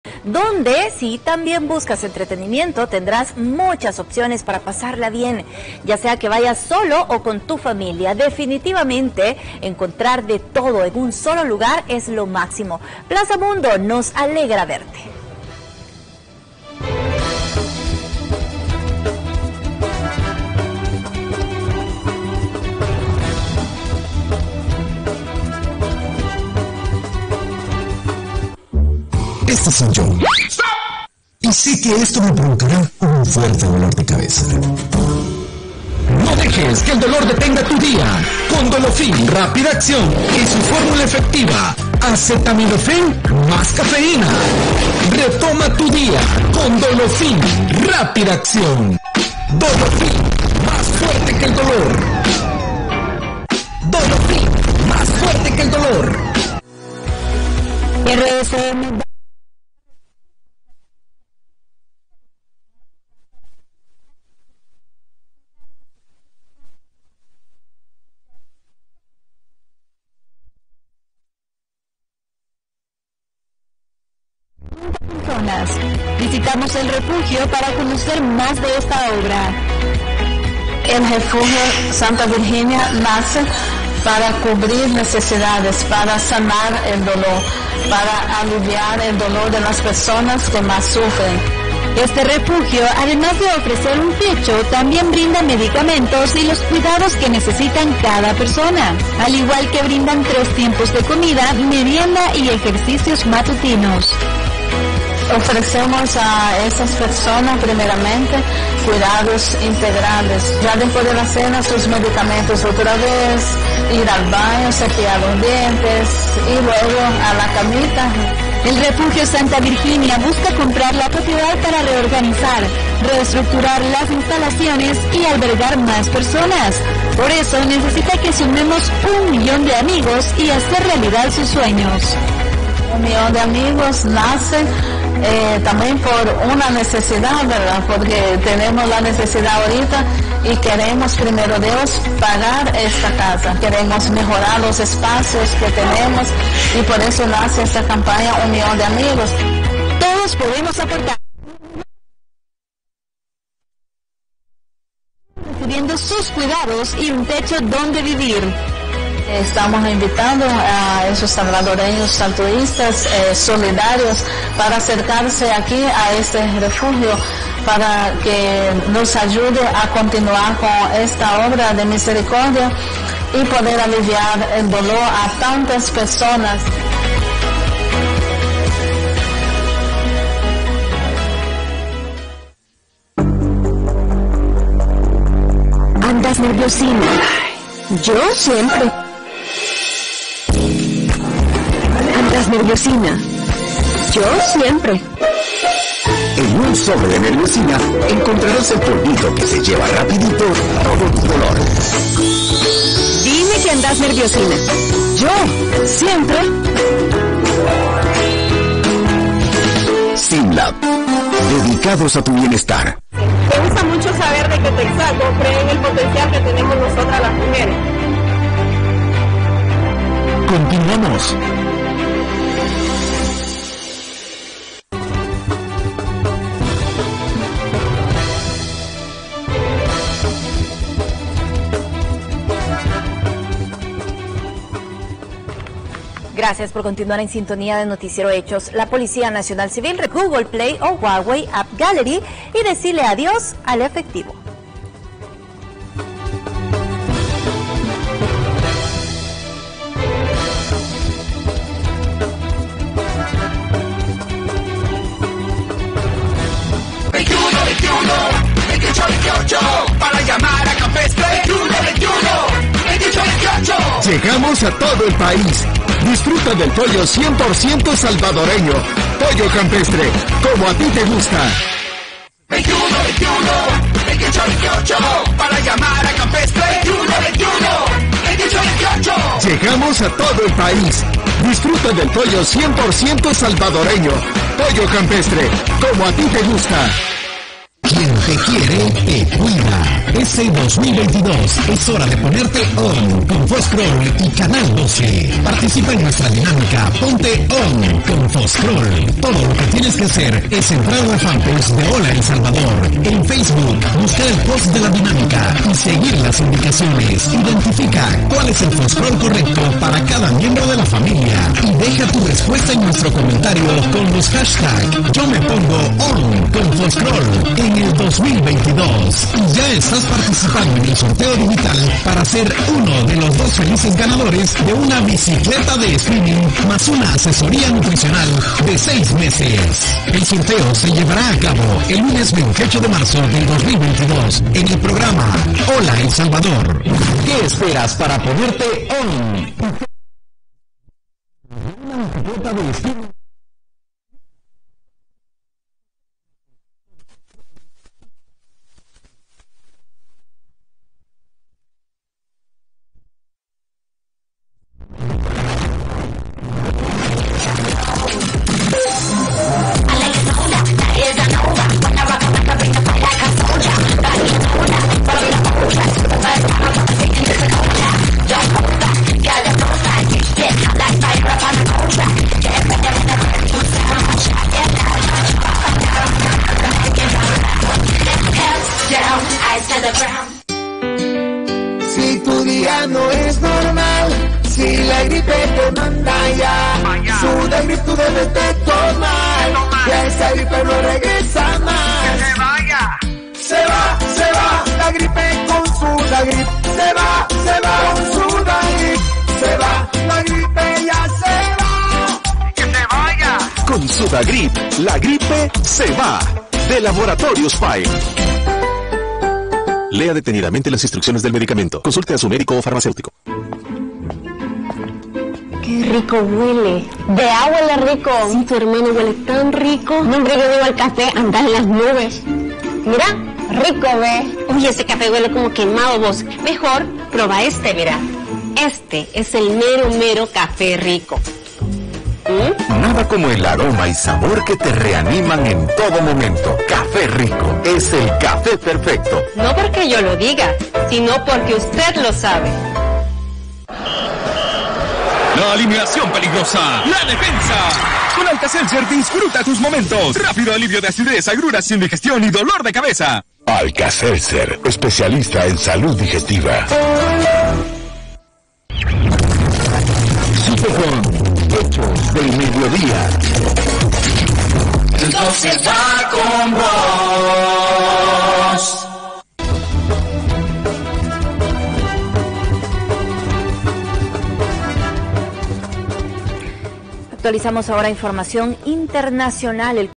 Tanda Comercial Canal 12 RSM (+Pifie)(Marzo 2022)
La falla solo ocurrio en el stream de play go! en la tv estaba normal